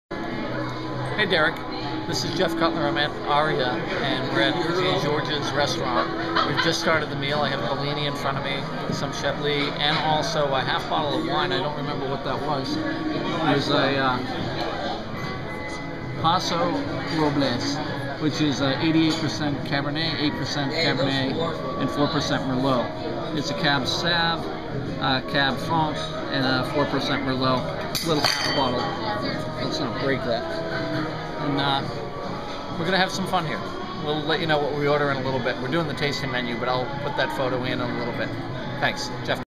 JeanGeorgesIntro_AriaLV
This is the beginning of our meal at Jean Georges Steakhouse at the Aria Hotel in Las Vegas. Enjoyed a rocktastic dinner there in January 2011 with Derek Wilmot. We were guests of the hotel and the restaurant fed us off the tasting menu - fabulous. It could be considered a scouting mission - great food. Kevin Palmer made the intro to the restaurant for us and should be commended and thanked.